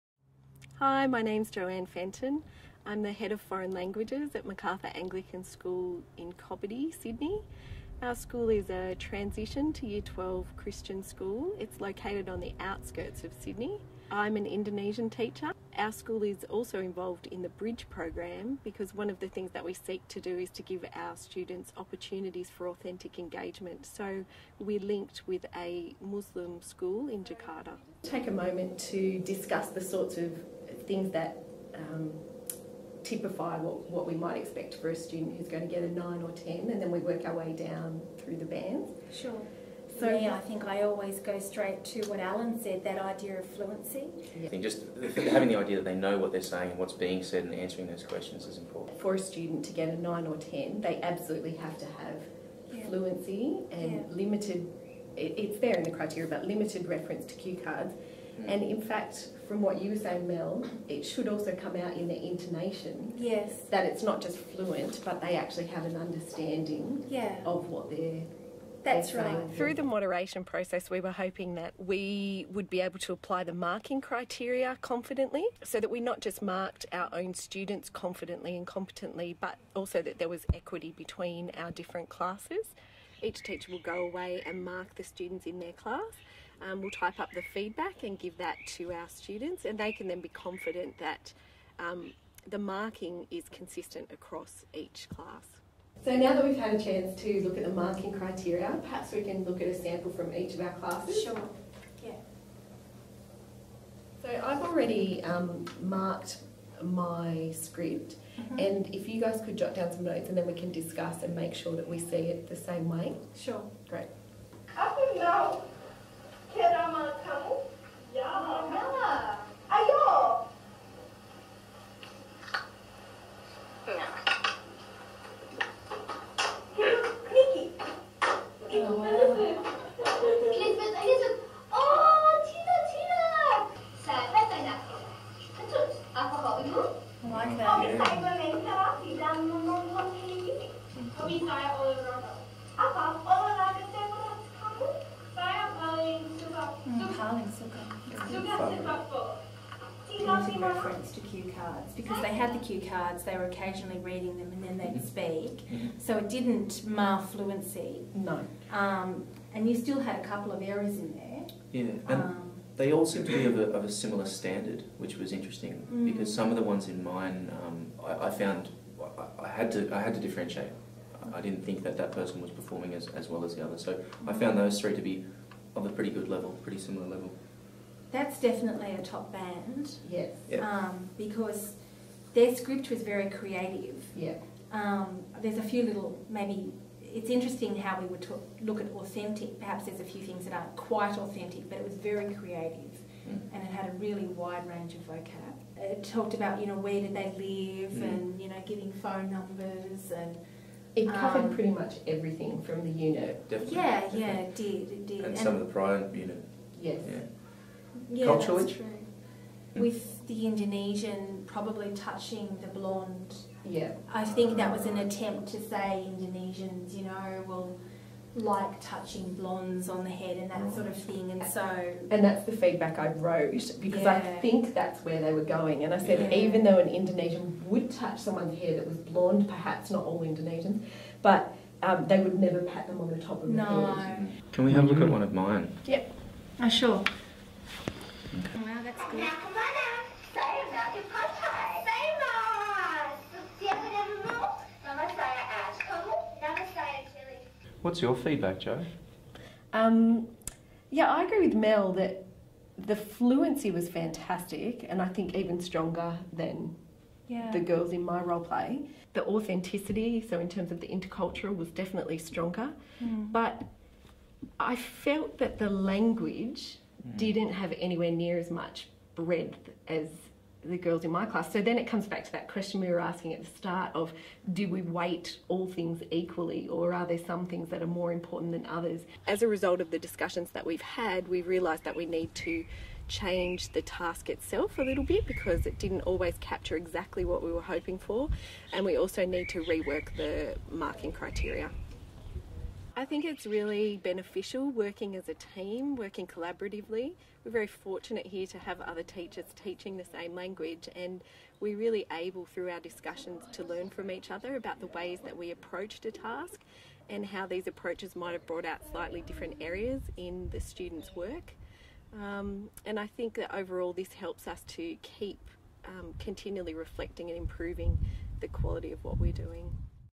Moderating language assessment
Career Stage - Highly Accomplished
Main Focus area - 5.3
School - Macarthur Anglican School (Secondary)

A teacher leads a moderation activity with two colleagues in order to ensure that consistent and comparable judgments about student learning are made to meet curriculum and school requirements. She leads two colleagues through an assessment moderation meeting for a recent Indonesian assessment task. Each teacher has taught a year 8 class and undertaken a common assessment task with their class. Using agreed marking criteria, the teacher leads her colleagues through an examination of some recorded role-plays in order to reach agreed understandings about how the criteria should be applied within the assessment moderation.